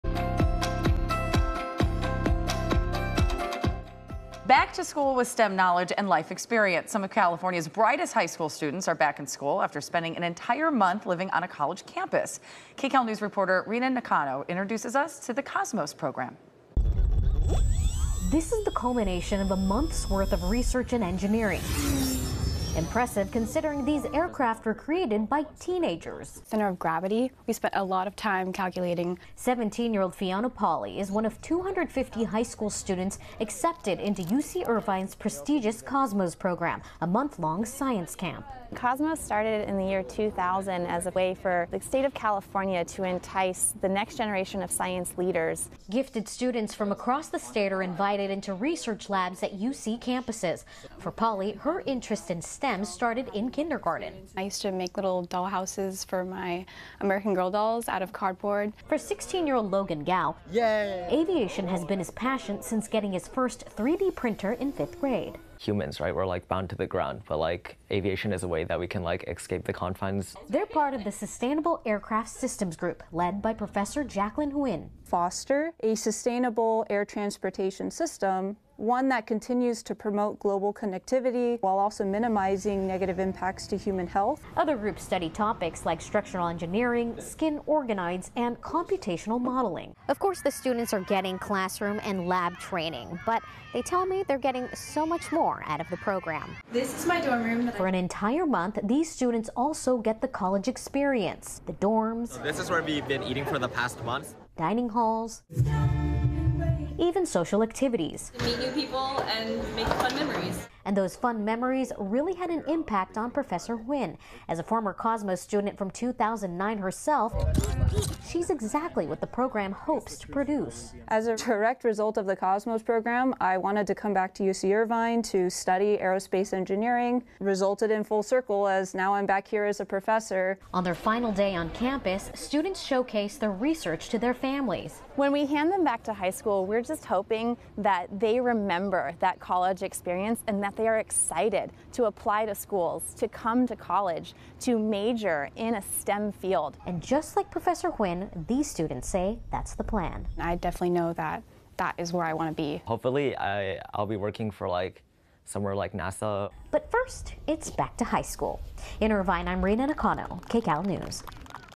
Back to school with stem knowledge and life experience
Some of California's brightest high school students are back in school after spending a month living on a college campus. KCAL's Rina Nakano shows us the Cosmos Program.